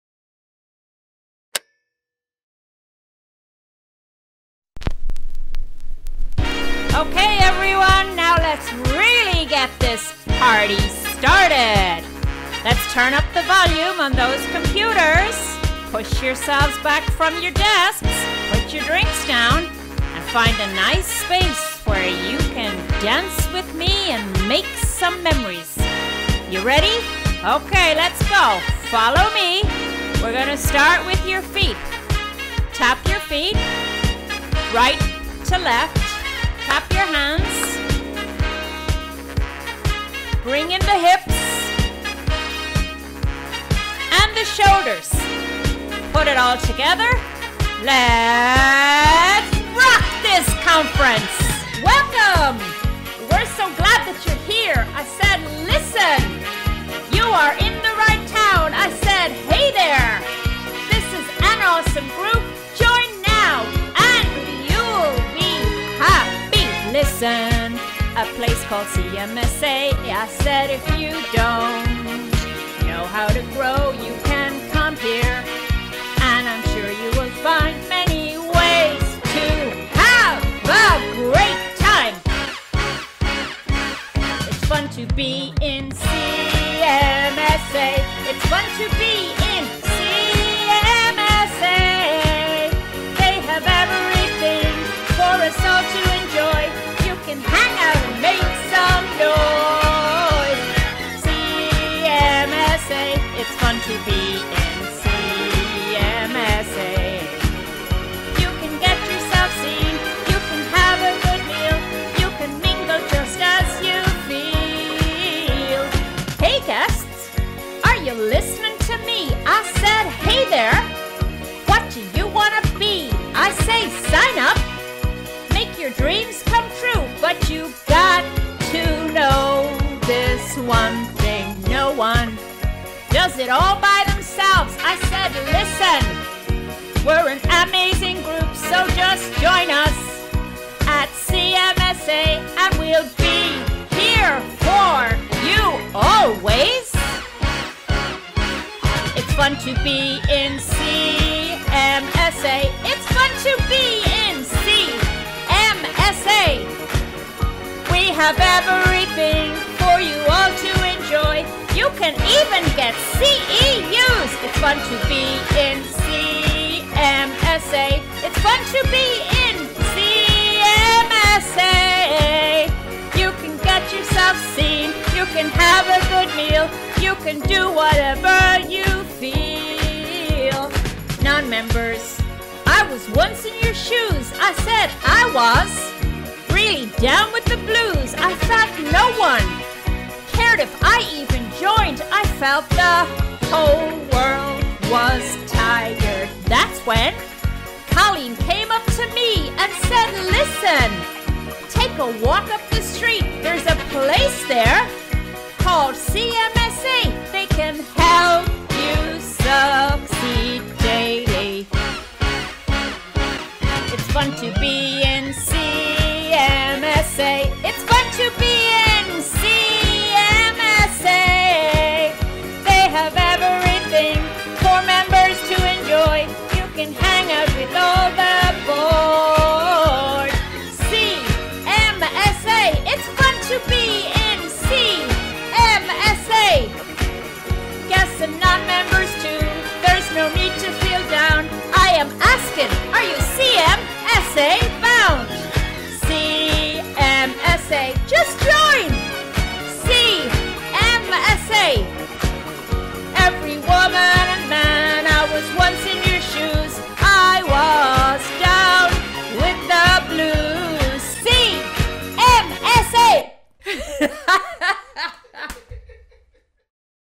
Aishling Music Presentation - CMSA: CHICAGO VIRTUAL CONFERENCE
The CSMA - CHICAGO VIRTUAL CONFERENCE was an extremely lively and successful event! I know how to have fun, create laughter, and make conference attendees' experiences memorable moments.

Who says virtual conferences can't be fun? Special shout out to Zee my very talented videographer who does all my work! He saw my vision for this presentation and made it come to life! Such fun!!

CMSA Chicago rocks!!!
LITERALLY!!